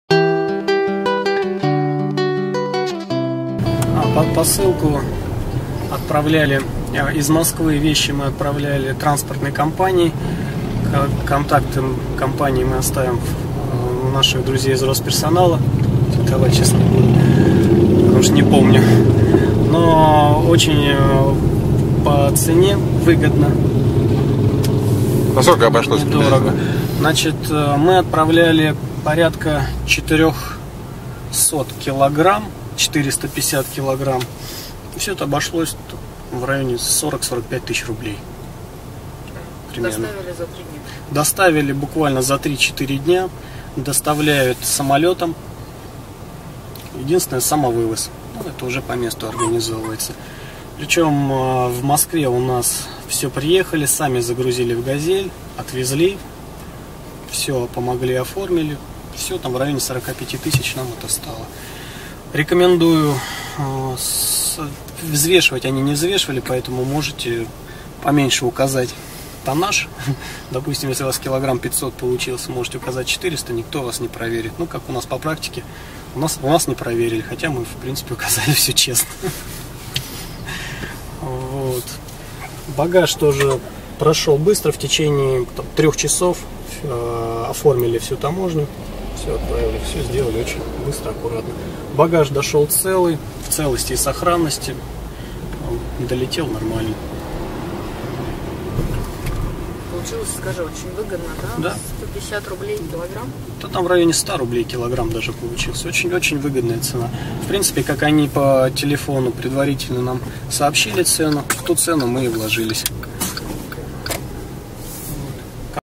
Рассказ иммигранта. Перевозка вещей в Австралию
Рассказ иммигранта о перевозке вещей из Москвы в Мельбурн, Австралия, и советы: особенности, хитрости, сроки и цены.